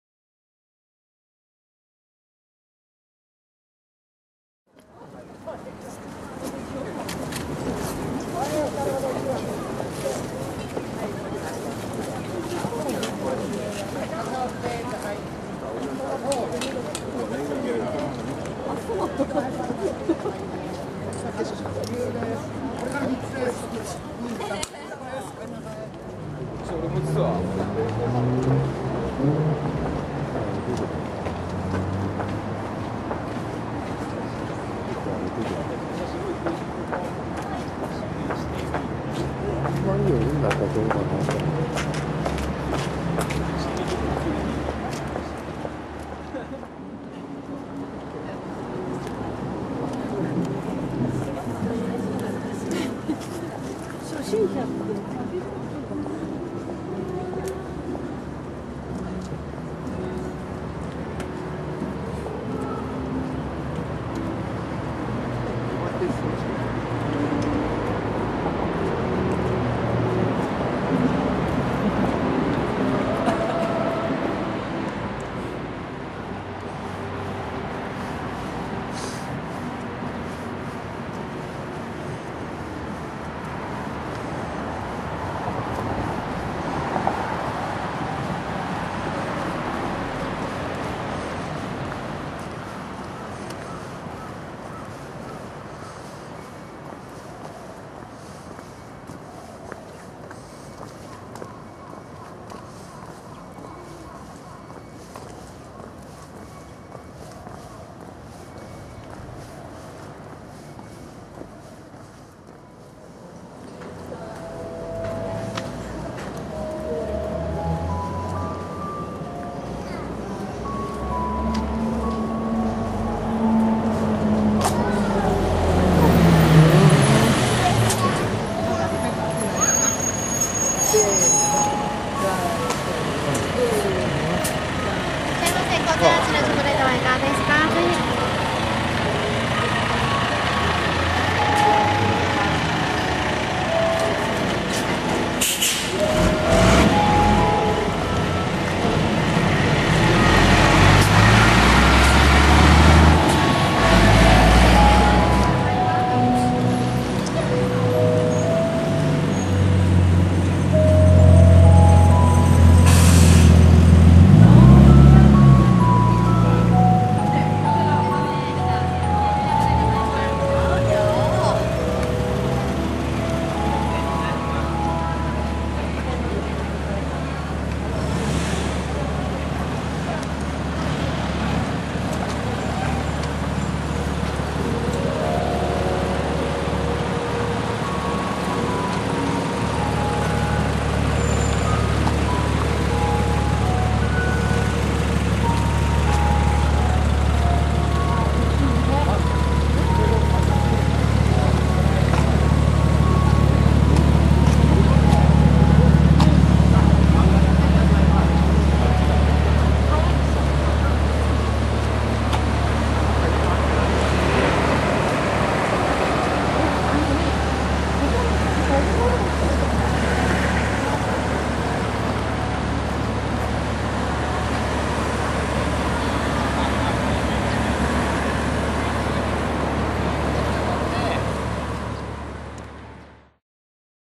小樽運河とメルヘン交差点２０１２
一昨年の春に小樽に訪れた時の映像です。